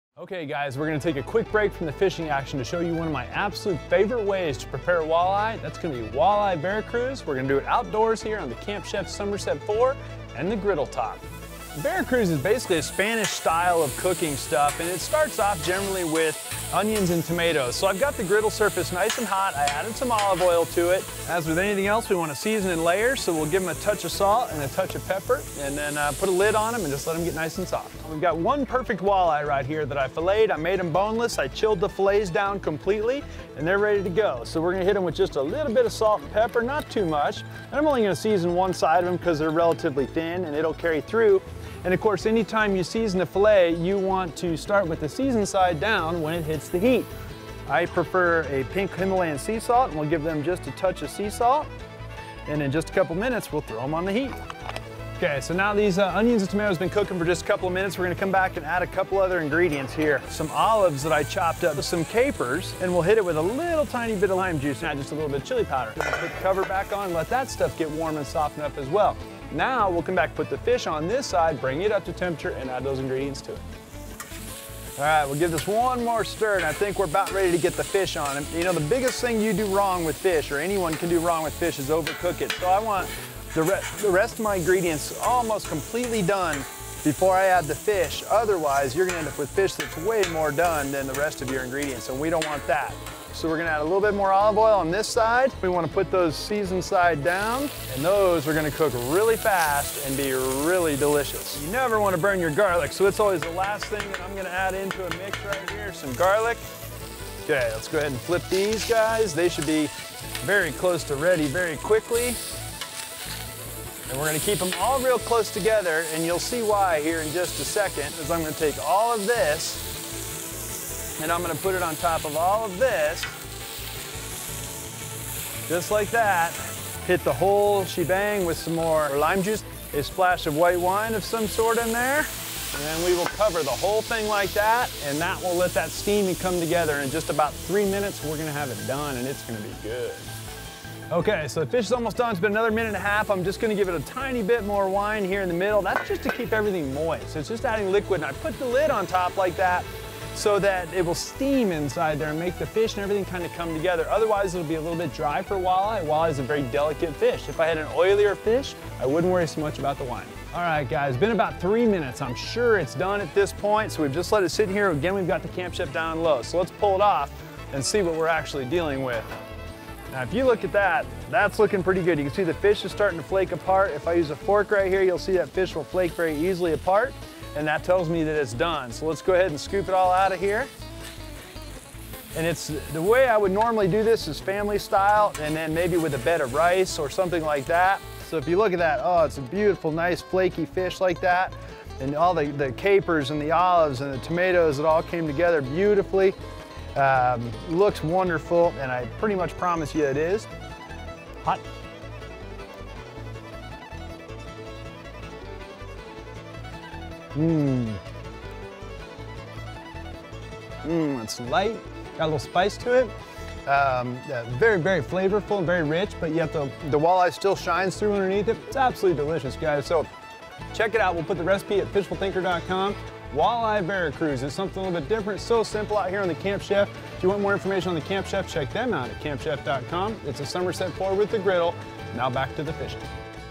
Vera Cruz style walleye on the Camp Chef griddle
This is a very tasty and quick way to cook just about any kind of fish fillet. The Camp Chef griddle is perfect for it and cooking outside not makes it taste better, but keeps the mess and smell out of your house!Check out the stove, griddle, and more